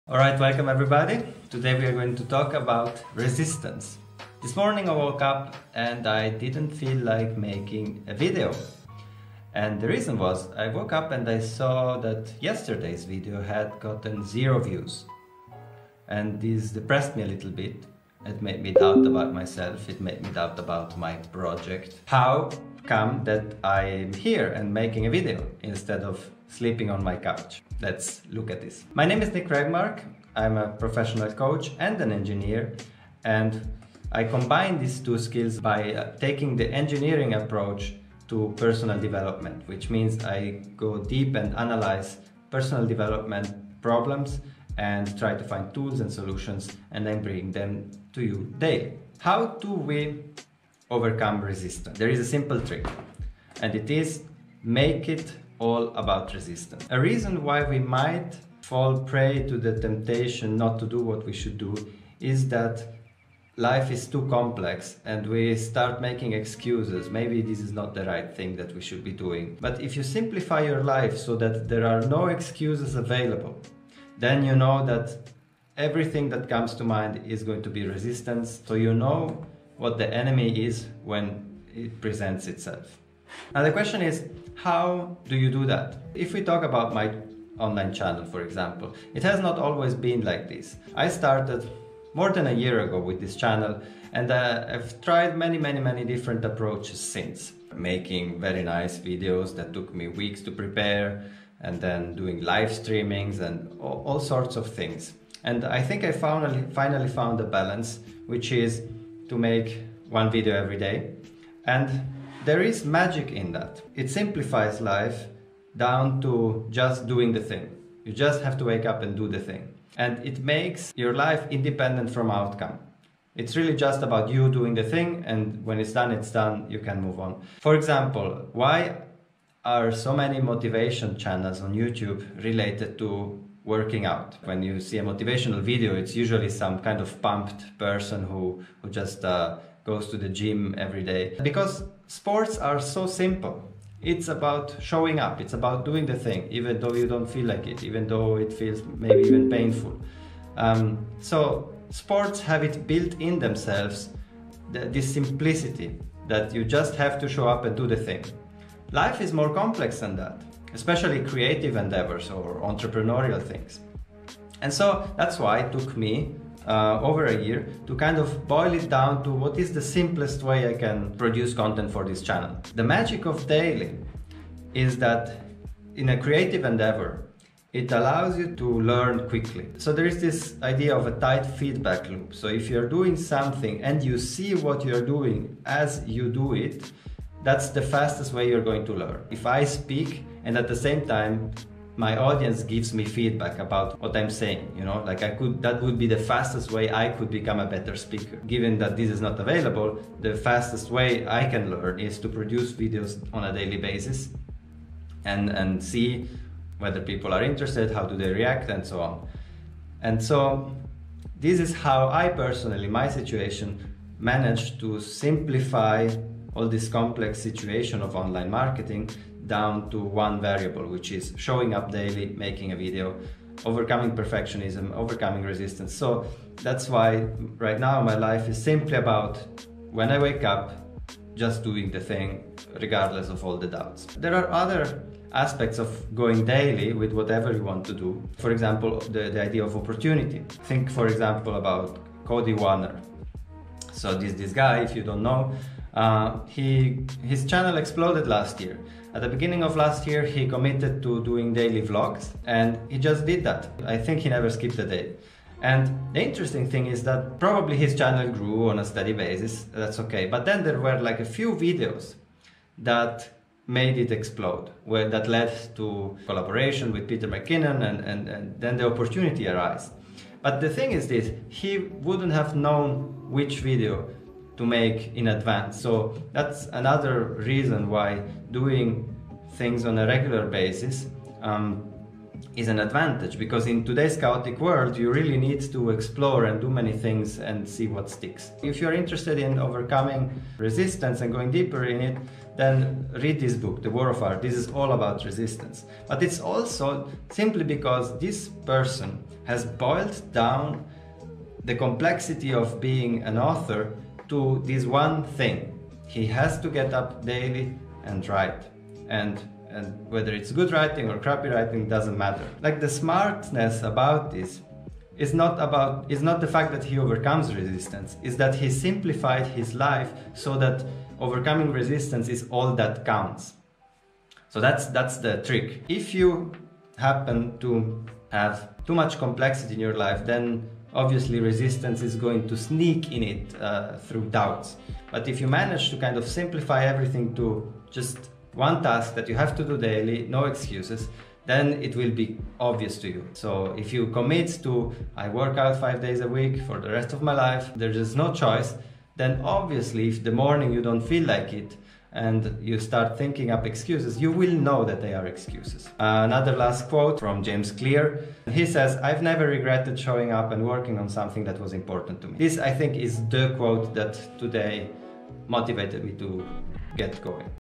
The Counterintuitive Trick to Overcoming Resistance (+ Thoughts about R Cody Wanner)
Self-doubts, anxiety, mental blocks. How do you overcome resistance on a daily basis? It's not what you think.